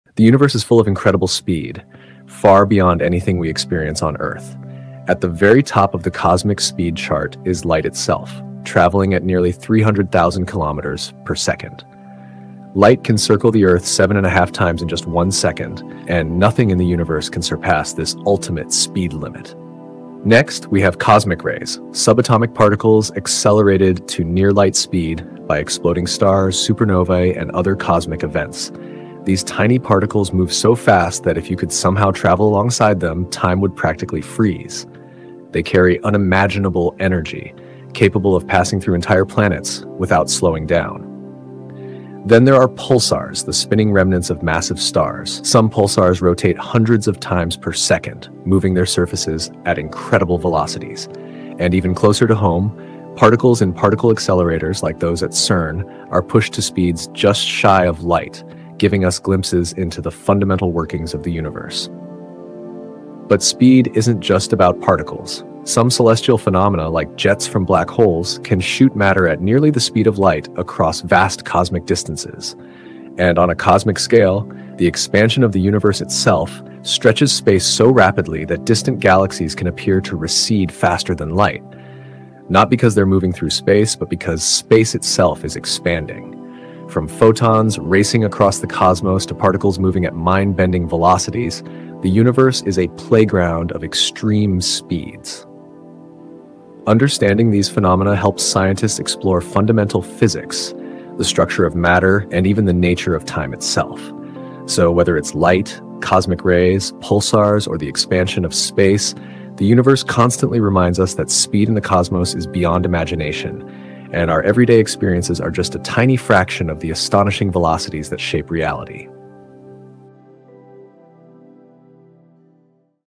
Fastest Things in the Universe Explained! ⚡🌌 | Scio-Eureka
From light itself to mysterious cosmic phenomena, the universe is full of mind-blowing speeds! 🚀 In this Short, we explore the fastest things in existence and how they push the limits of physics. Prepare to be amazed by the incredible pace of the cosmos!